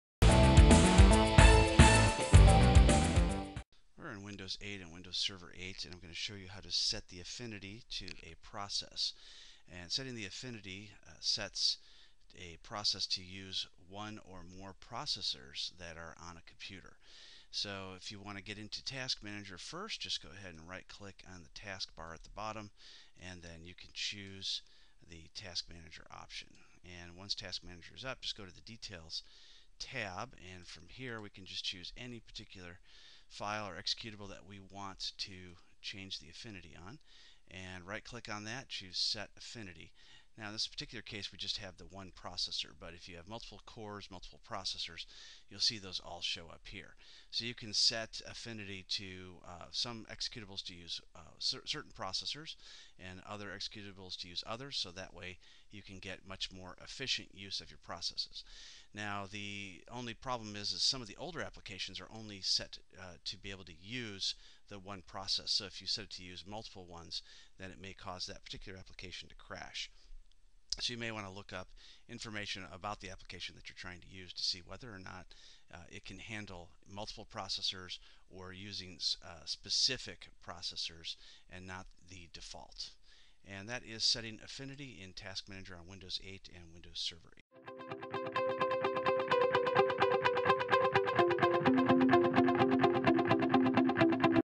How to set the Affinity using Task Manager in Microsoft Windows 8
Author and talk show host Robert McMillen explains how to set the Affinity using Task Manager in Microsoft Windows 8.